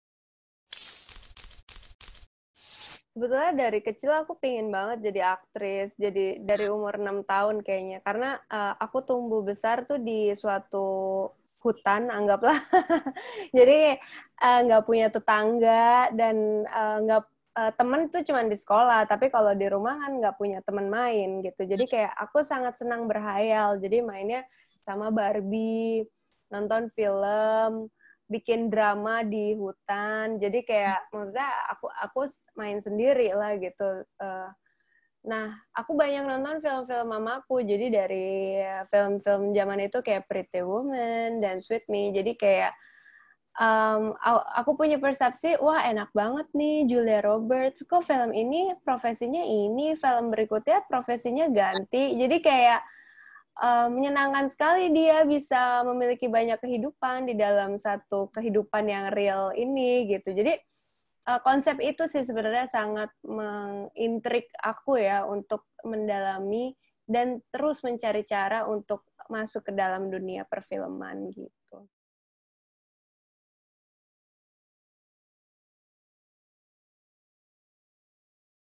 Sejak Kecil Asmara Abigail Bercita cita Menjadi Aktris
Asmara Abigail mengaku sejak kecil sudah ingin menjadi aktris. Sebab saat kecil, Asmara tinggal di rumah yang tidak memiliki tetangga. Sehingga ia tak memiliki teman untuk bermain di rumah.

Asmara kecil menghabiskan hari-harinya dengan bermain barbie. Dari situlah Asmara mulai senang berkhayal.

Selain itu sehari-hari Asmara juga senang menonton film koleksi ibunya. Ketika itu Asmara melihat Julia Roberts yang bisa memiliki beragam peran dalam hidupnya. Dari situlah muncul impian untuk bisa bermain film.

Sumber foto: MI/PIUS ERLANGGA